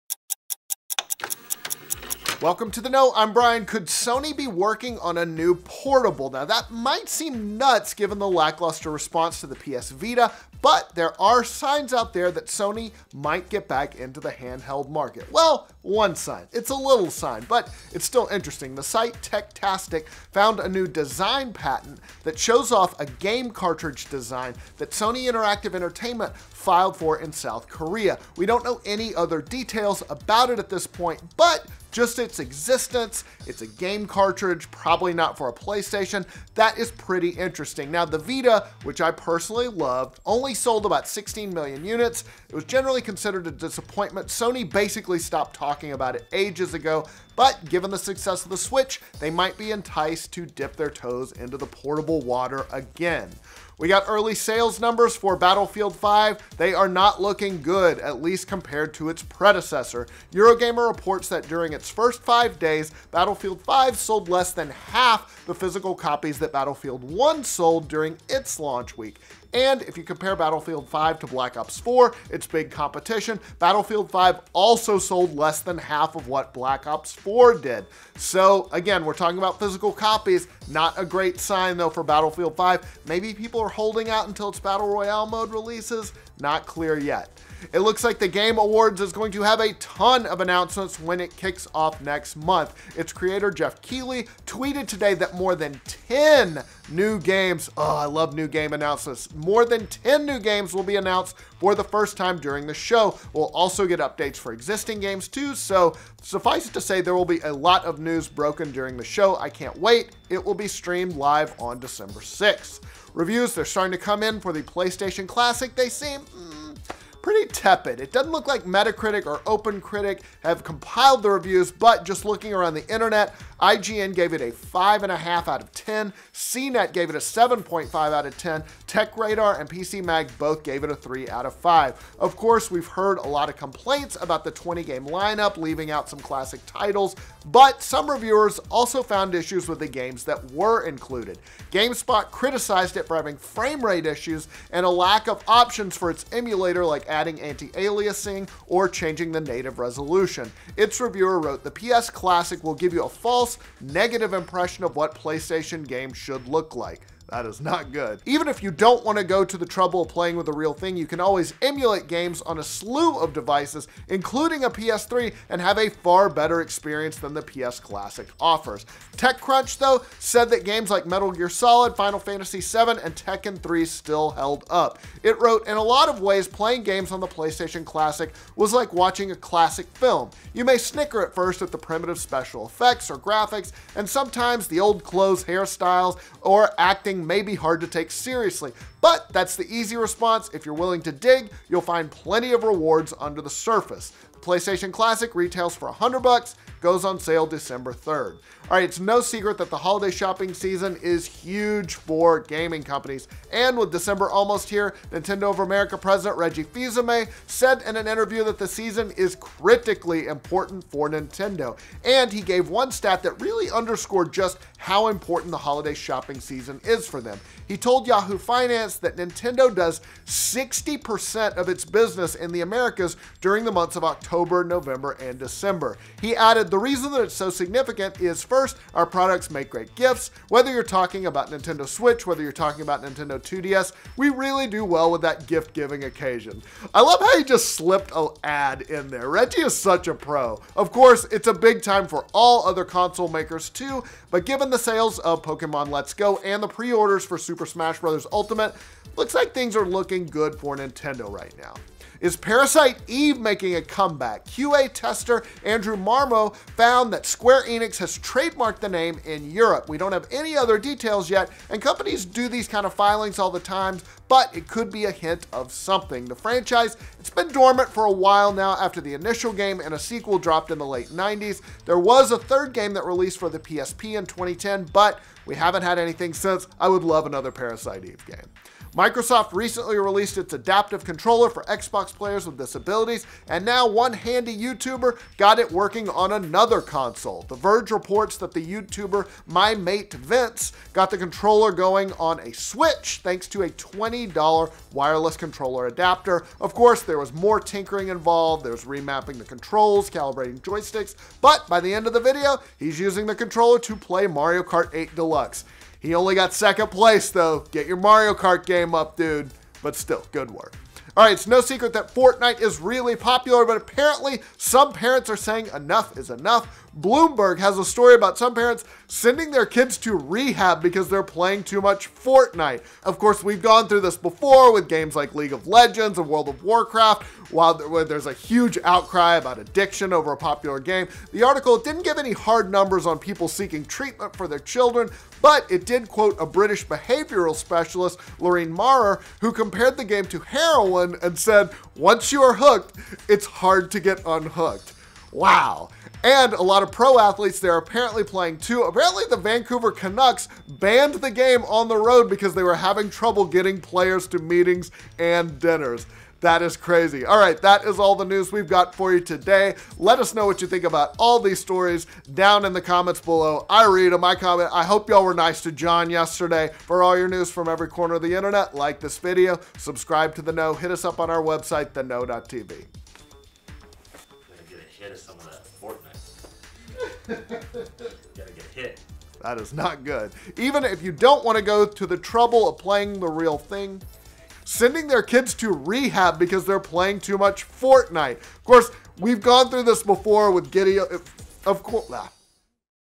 Sony Making Vita 2? + New Parasite Eve Trademark + Kids In Rehab Over Fortnite
A new patent filed by Sony shows a cartridge design that could mean we'll be getting a new portable Playstation, Square Enix has filed for a trademark in the UK for something related to Parasite Eve, parents are sending their kids to rehab because Fortnite is just too addictive, and more!

Hosted By: Brian Gaar
Written By: Brian Gaar
Edited By: Kdin Jenzen